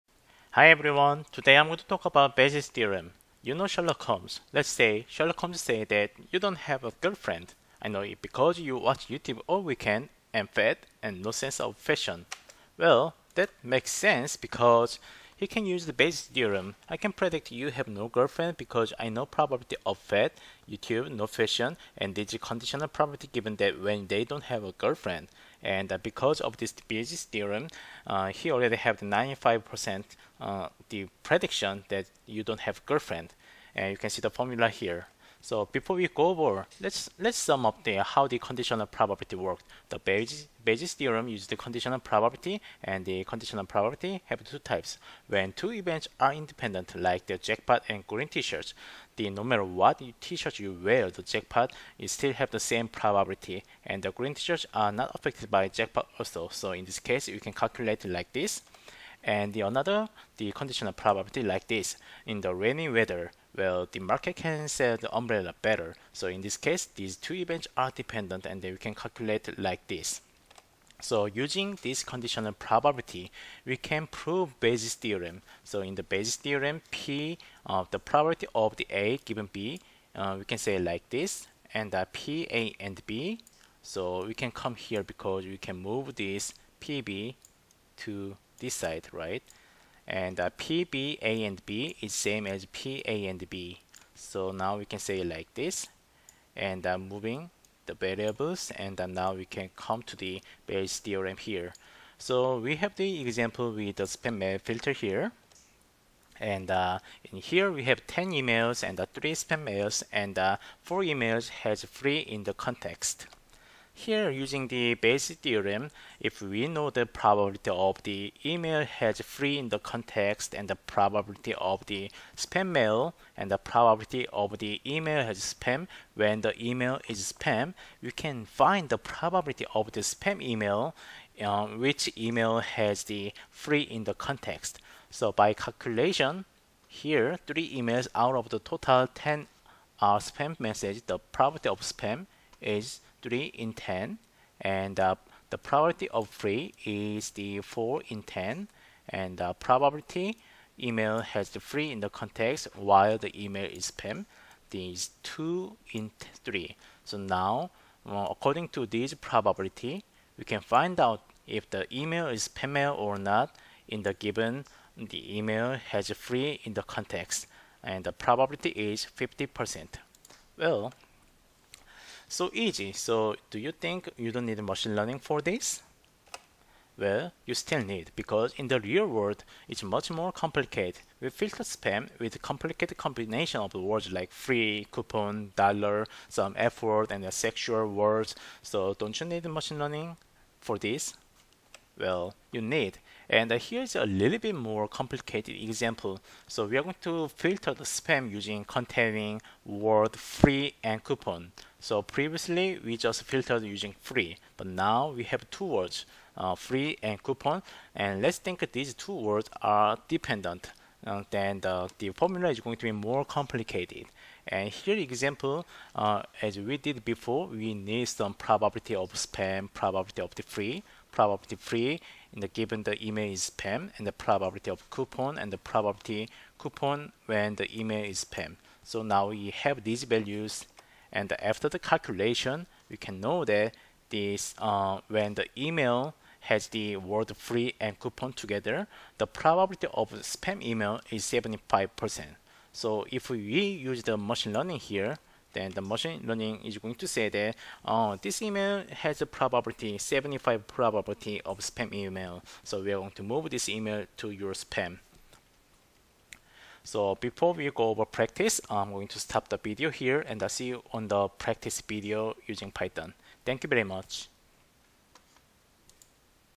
[Machine Learning] Naive Bayes Classifier (2/2) - Bayes Theorem
Describe Bayes Theorem in 5 minutes and get ready for programming.
Subtitle (English) is ready, please click CC button on youtube video for subtitle.